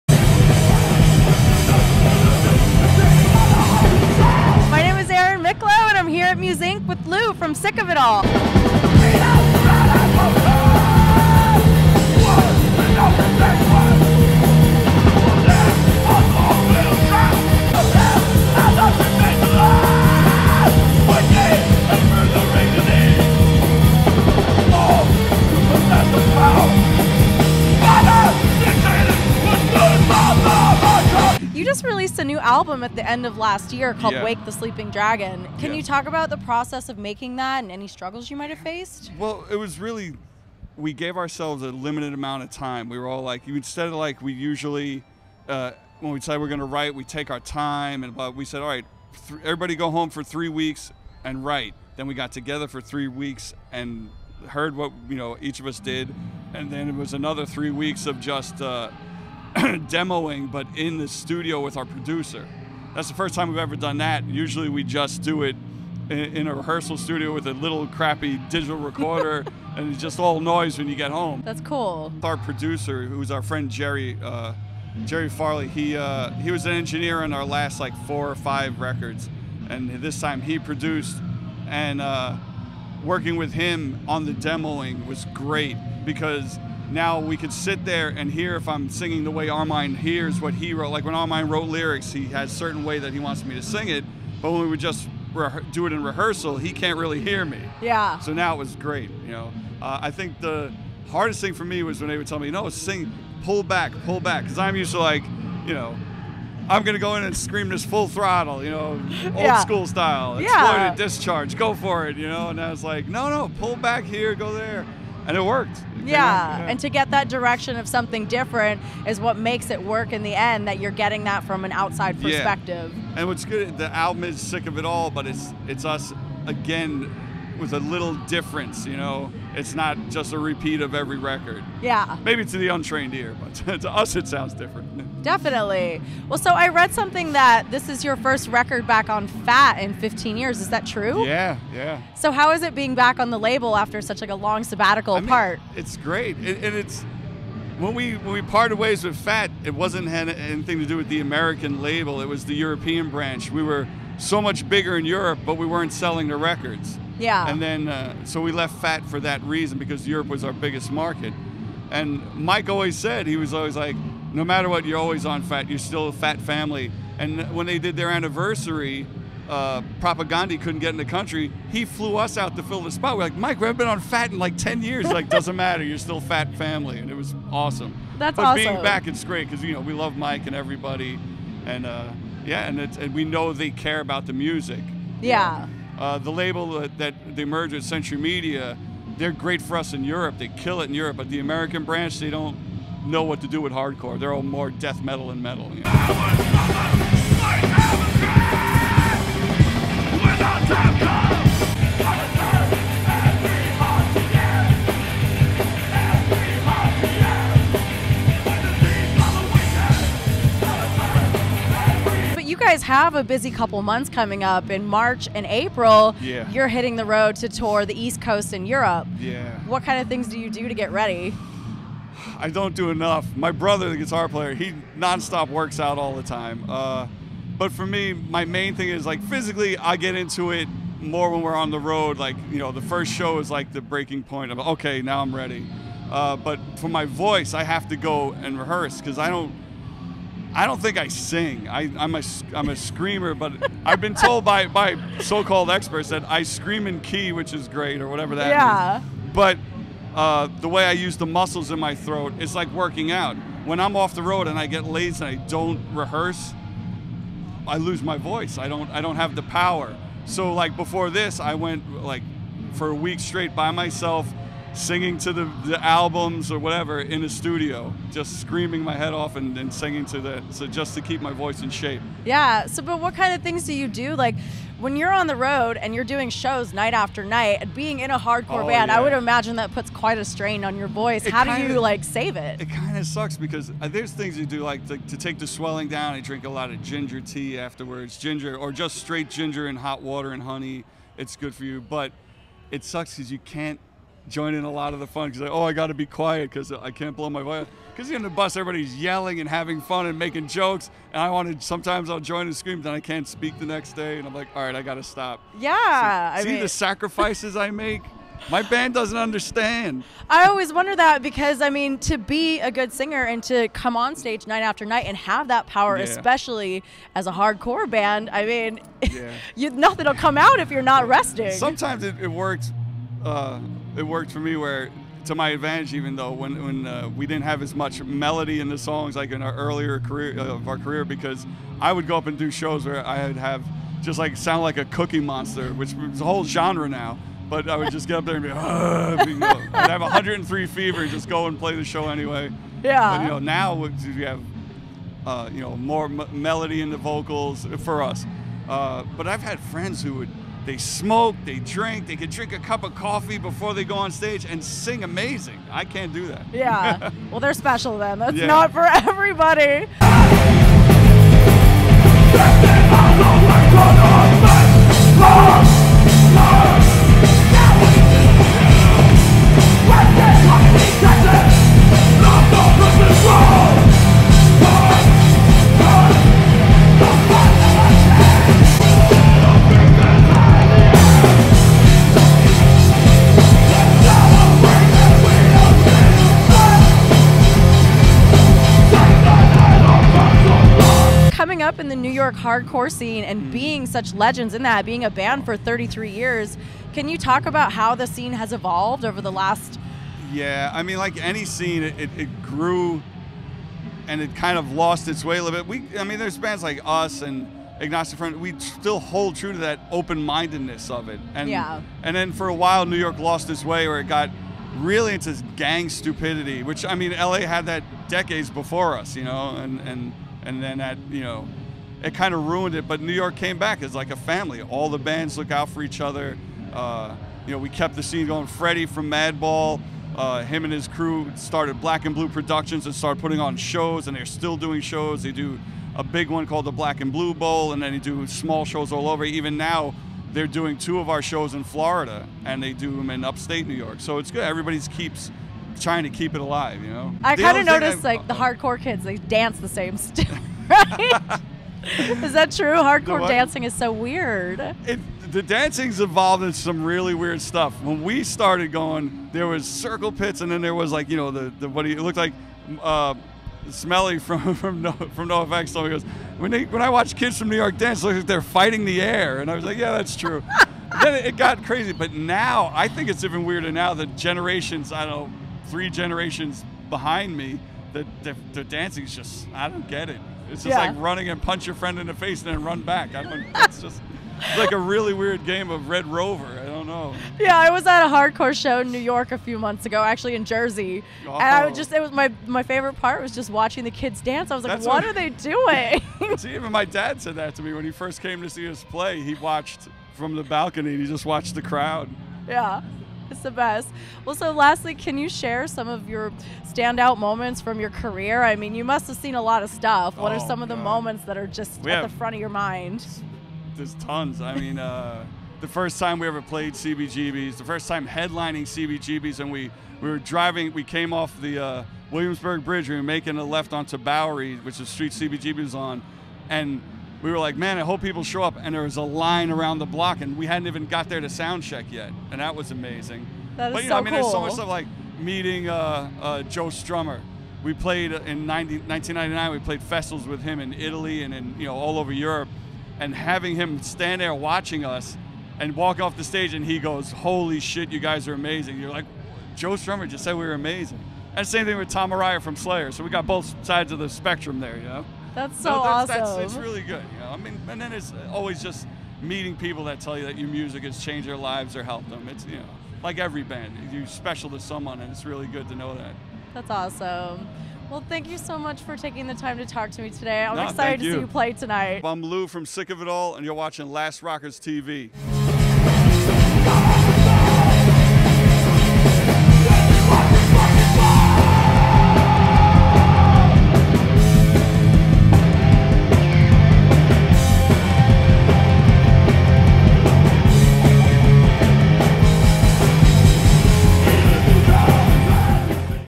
SICK OF IT ALL (NY Hardcore) interview: NEW ALBUM, BACK ON FAT WRECK CHORDS AFTER 15 YEARS
Lou Koller of Sick of It All talks with Erin Micklow for Last Rockers TV at Musink Fest 2019 in Costa Mesa, CA!